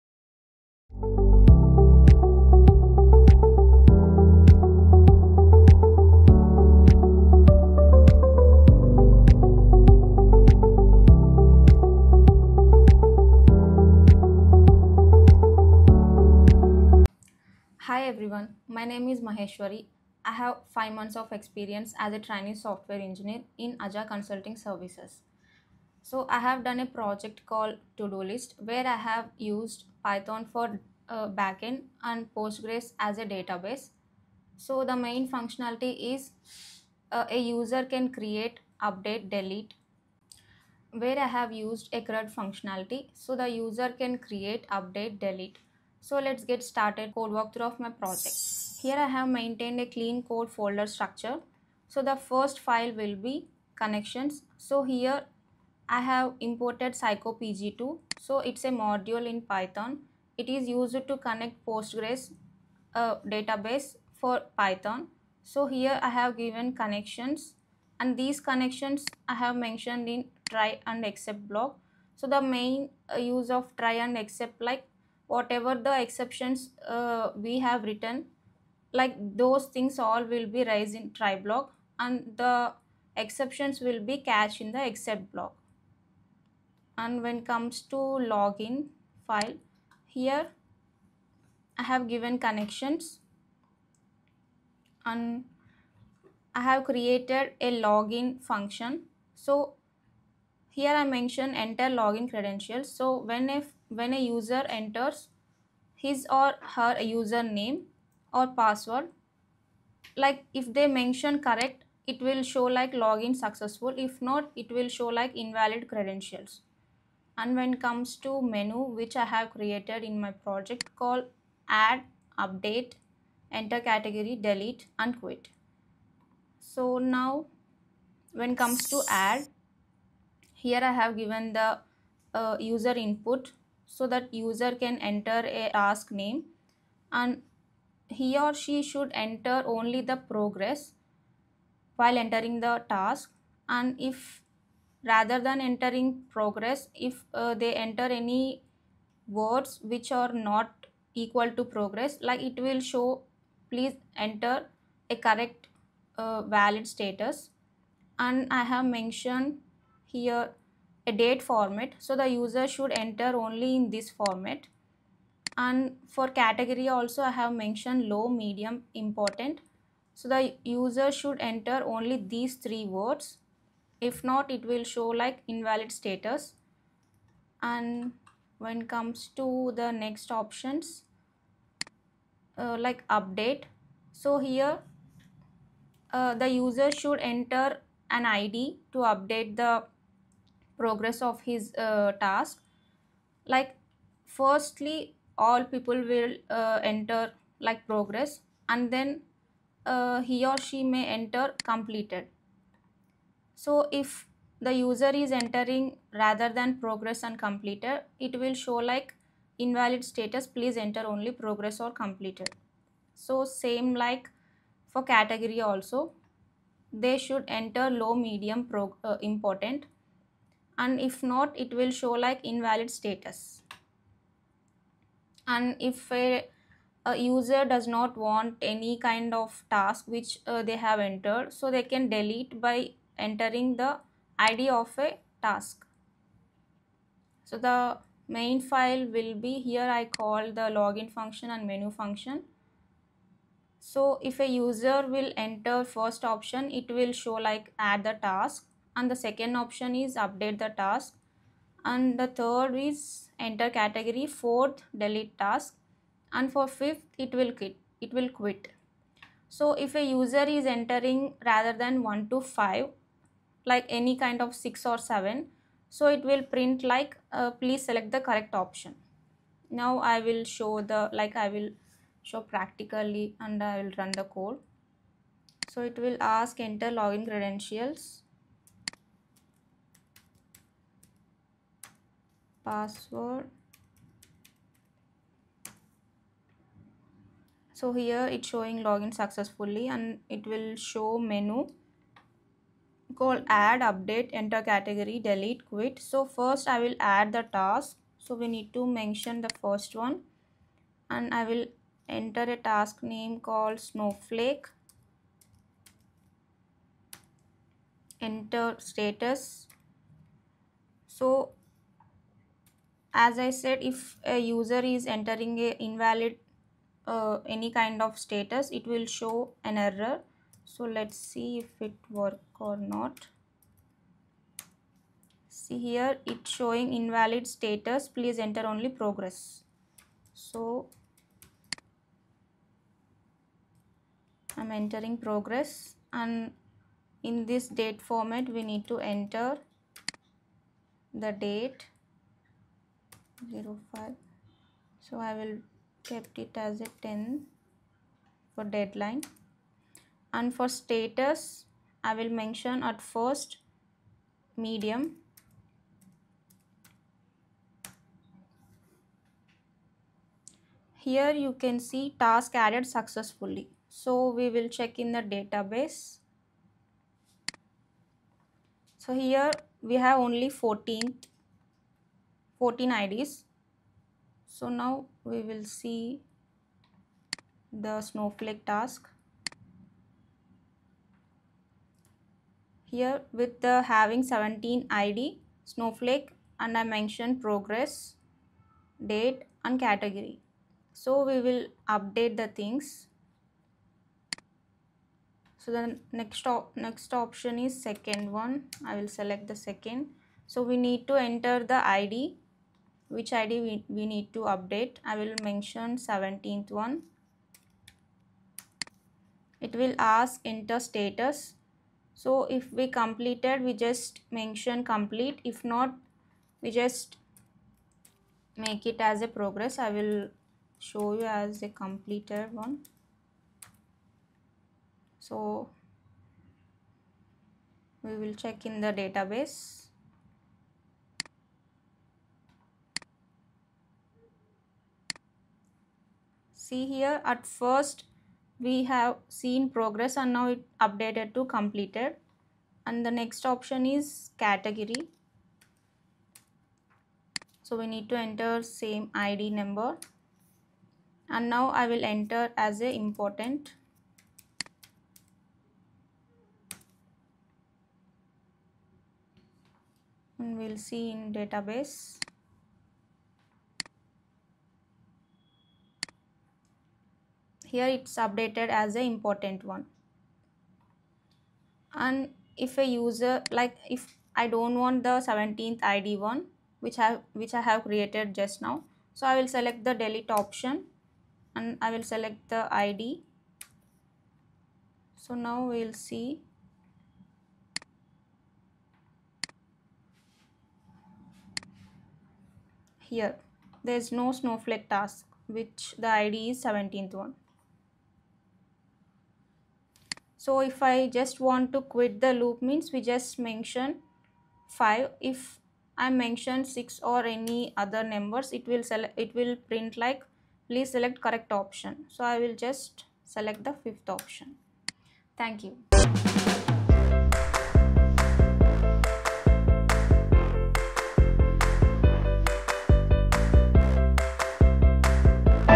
Code Walk Thru - By Maheswari Devi Nagasamudram - Python Developer @ Aja Coding Tutor
In this episode of Code Walk Thru, Maheswari Devi Nagasamudram, Python Developer at Aja, takes you behind the scenes of her development process. Watch as she breaks down real-world code, shares her approach to solving problems, and walks through clean, efficient Python solutions.

Whether you're a beginner trying to understand how professionals think through code or an experienced developer looking to refine your practices, this walkthrough is packed with practical insights and hands-on learning.